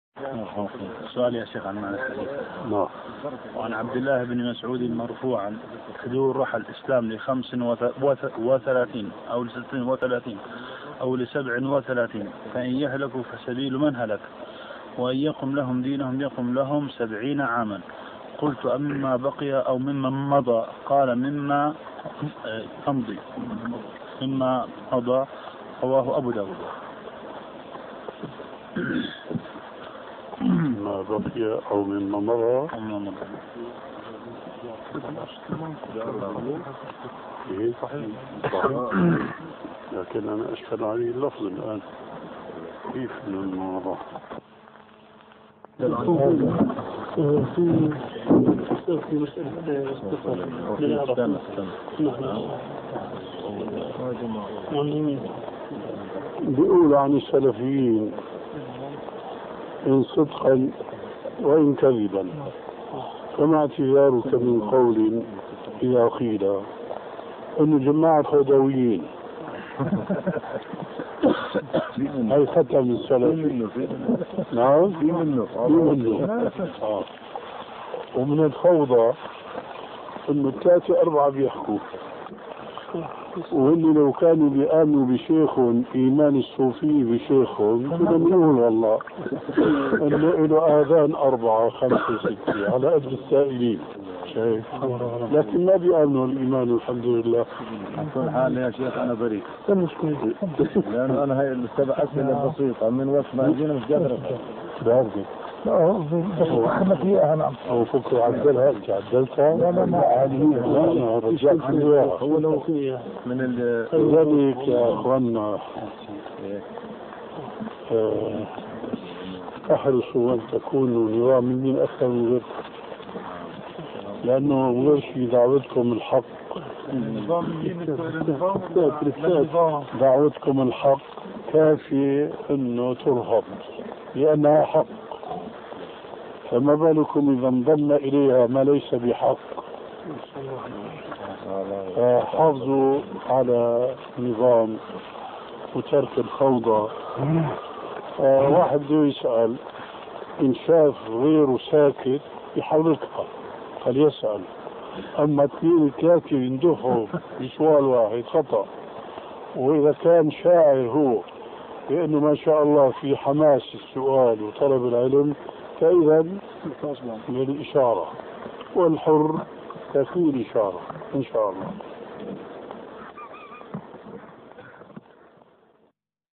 13-23 عن ابن مسعود مرفوعاً ( تدور رحى الإسلام بخمس وثلاثين أو ست وثلاثين أو سبع وثلاثين ) الألباني
❓عن عبدالله بن مسعود مرفوعاً : ( تدور رحى الإسلام بخمس وثلاثين أو ست وثلاثين أو سبع وثلاثين فإن يهلكوا فسبيل من هلك وإن يقم لهم دينهم يقم لهم سبعين عاماً قلت أمما بقي أو مما مضى قال : مما مضى ) رواه أبو داود ما صحة الحديث .؟

السائل : سؤال عن معنى الحديث : وعن عبد الله بن مسعود مرفوعاً ( تدور رحى الإسلام لخمسٍ وثلاثين أو لستٍ وثلاثين أو لسبعٍ وثلاثين فإن يهلكوا فسبيل من هلك وإن يقم لهم دينهم يقم لهم سبعين عاماً قلت أمن بقي أو من مضى؟ قال مما مضى ) رواه أبو داود.
الشيخ : مما بقي أو مما مضى
السائل : نعم
الحلبي : صحيح
الشيخ : لكن أنا أشكل عليَّ اللفظ الآن كيف مما مضى.
الشيخ : بقولوا عن السلفيين إن صدقاً وإن كذباً فما اعتذارك من قولٍ إذا قيل هم جماعة فوضويين
يضحك الطلبة
السائل : بقول في منه
الشيخ : نعم
السائل : في منهُ
الشيخ : في منه آه ومن الفوضى ثلاثة أربعة بحكوا وهم إذا كانوا بآمنوا بشيخهم إيمان الصوفية بشيخهم كنا بتقول و الله له اذان أربعة خمسة ستة على عدد السائلين. شايف لكن مابآمنوا هذا الإيمان الحمد لله
السائل : على كل حال ياشيخ أنا برئ لأنه أنا هذه الأسئلة البسيطة من وقت ماجئنا ...
الشيخ : ما عدلتها ... يا أخوانا احرصوا على أن تكونوا نظاميين أكثر من غيركم لأنه دعوتكم الحق وكافية أن تظهر لأنه حق فما بالكم إذا انضم إليها ماليس بحق فحافظوا على النظام وترك الفوضى فإذا واحد يريد أن يسأل إن شاف غيره ساكت فليحركها فيسال أما اثنان ثلاثة يندفعوا بسؤال واحد خطأ وإذا كان هو شاعر هو أنه ما شاء الله في حماس السؤال وطلب العلم فإذاً بالإشارة والحر تكفيه الإشارة. إن شاء الله .

 أعــــده 📚
📚ابو_حامد_يوسف_شومان 🌱🍒ة*
*🌹فالـدال عـلى الخــير كـفاعـله